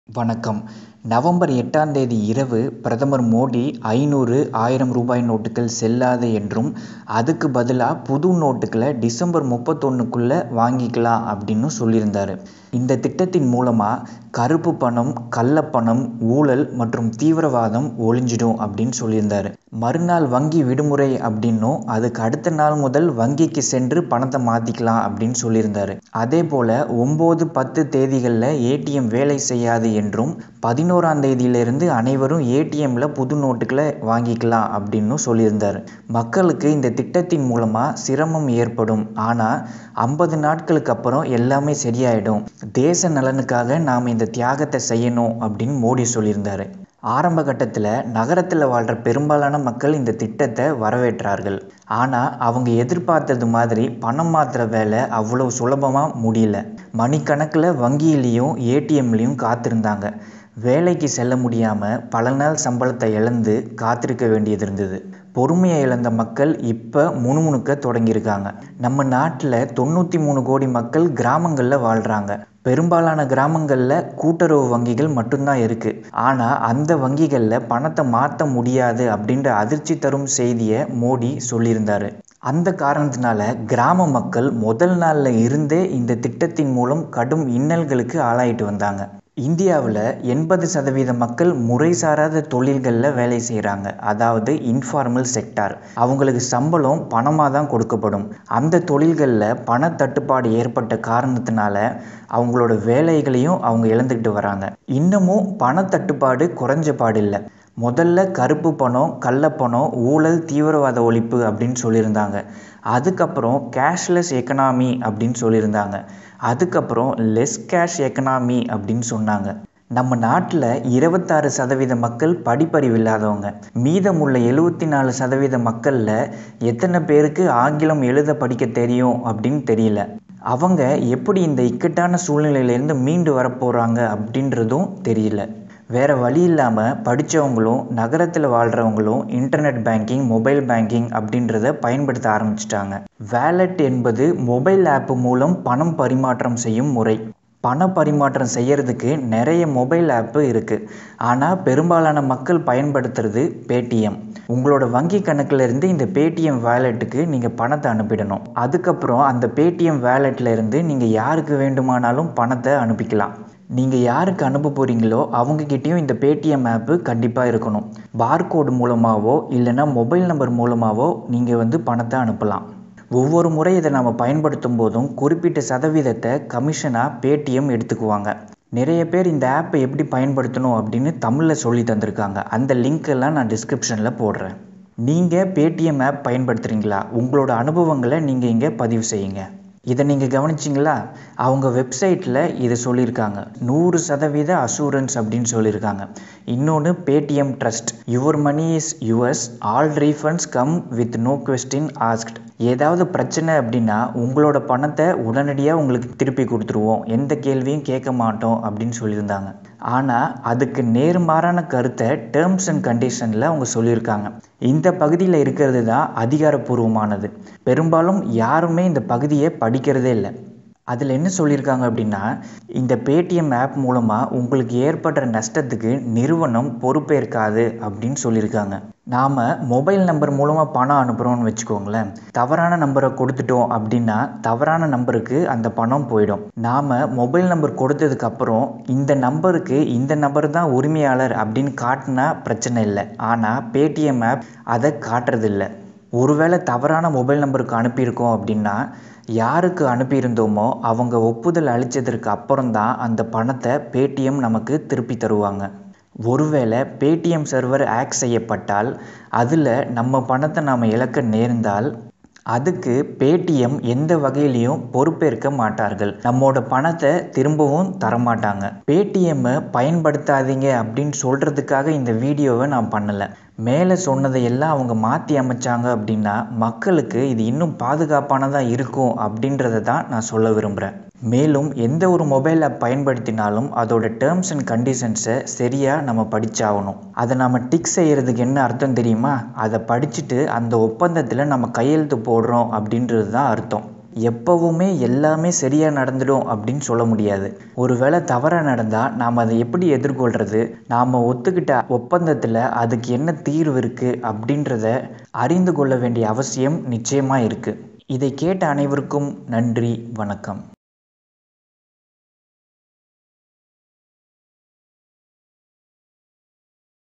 Is Paytm app not safe? | Paytm app ஆபத்தானதா? உஷார்
On 8th October 08:30 pm, Modi announced that 500, 1000 rupee notes will no longer be valid after 8th october midnight . He told that India has to do this to stop black money and fake money circulation. Everyday new announcements coming from Reserve bank of India.

Modi asked people to go digital(Internet banking, card payment, mobile payment). Mobile wallet is used to send or receive money in mobile. PayTm is used by majority of the people. Is PAytm safe? Please check out the video and share your thoughts.